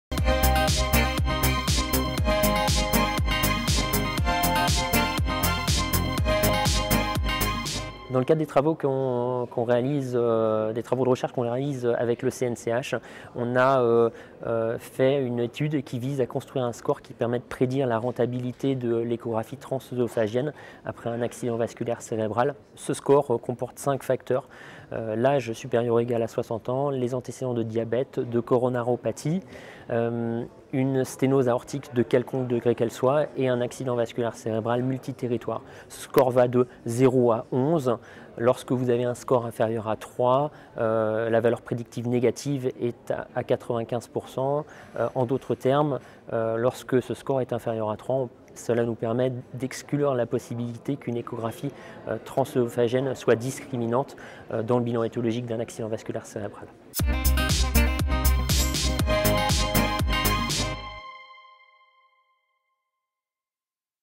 Hot Line JE SFC 2018 - L'étude ADAM-C score
Clément Charbonnel cardiologue au CHG de Versailles, fait la synthèse de cette étude menée dans le cadre des travaux de recherche du Collège National des Cardiologues Hospitaliers (CNCH),  qui permettait de prédire la rentabilité d'une échographie transoesophagienne après un accident vasculaire cérébral. Cette étude a englobée 1500 patients dans 27 centres en France.